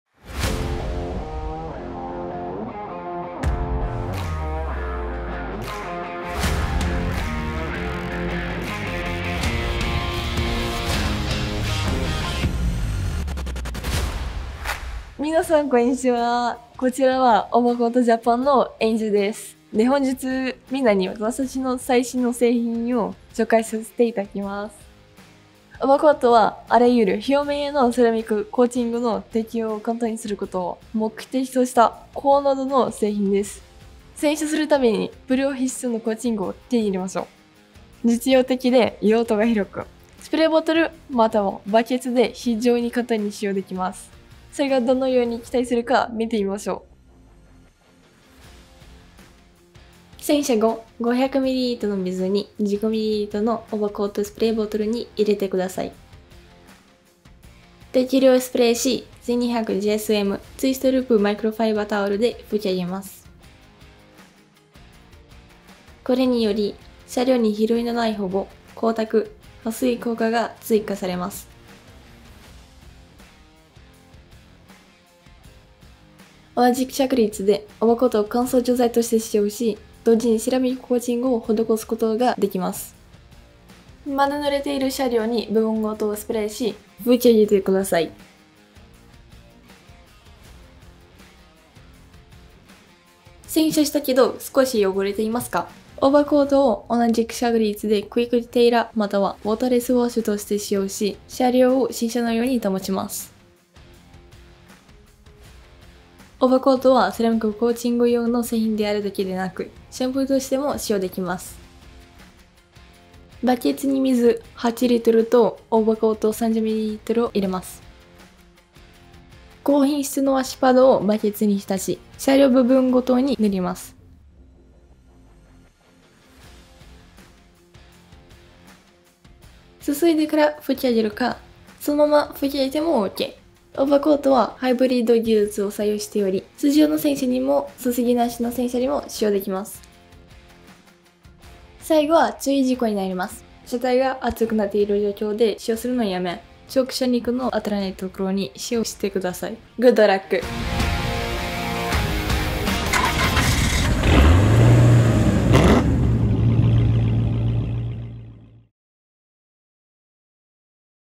【オーバーコート】ガイド｜使用方法
イノベーションはここにある。
見たことのないようなセラミック保護。 あなたの車のお手入れは、これまでになく簡単になる。

想像を絶するものをあなたの手に。

非常に簡単に適用できるプロ仕様のセラミック保護。
実用的で用途が広く、さまざまな用途に使用できます。
コストパフォーマンスに優れた高濃度。
ディテーリングの専門家や自動車愛好家向け 。

20年以上の経験によりできた、この製品は自動車界に革命をもたらす。

セラミックコーティング。
比類のない光沢、保護。

セラミックコーティングは、常に手間がかかり、適用が複雑なイメージ。
オーバーコートを使用すると、洗車のたびにプロ級のセラミック保護を得ることができる。
お好みの商品で洗車後、オーバーコートをシャンプー感覚で塗布するだけで、極上のセラミックコーティングが完成する。
非常に簡単でシンプルな方法で、比類のない光沢と保護を実現します。

ハイブリッド技術。
あらゆる方法でコーティングしよう。

オーバーコートは洗い流しても流さなくても構いません。 バケツ、噴霧器、ホースの有無にかかわらず。 オーバーコートは、自分の好みに合わせて自由に使用できる。

あなたが望むのは汎用性ですか？
オーバーコートは、ドライングエイドまたはクイックディテーラーとして使用可能。
車体、ガラス、タイヤ、ホイール、タイヤハウス、プラスチック、ゴム、ビニール、皮革、乾燥した表面または湿った表面に使用できる。

プレミアムで経済的。
使い方次第で30倍以上希釈できる高濃度製品です。

コーティングの適用
使用方法

スプレーボトル

コーティング
- スプレーボトルで500mlの水に15mlのオーバーコートを加えます。
- 車両部分ごとにスプレーして1200GSMマイクロファイバーツイストループタオルで拭き上げる。

ドライングエイド＋コーティング
- 洗車後濡れた状態で車両部分ごとにスプレーして1200GSMマイクロファイバーツイストループタオルで拭き上げる。

クイックディテーラー＋コーティング
- 洗車後、車両を拭いてから部分ごとにスプレーして1200GSMマイクロファイバーツイストループタオルで拭き上げる。

バケツ

コーティング
- バケツ8Lの水に30mlのオーバーコートを加えます。
- ウォッシュパッドをバケツの溶液に浸し塗布する。
- 塗布した箇所を洗い流して拭き上げる。もしくは、乾いた1200GSMマイクロファイバーツイストループタオルで拭き上げる。

*コーティング中のシミ防止として、部分箇所での使用をおすすめします。

overcoat.jp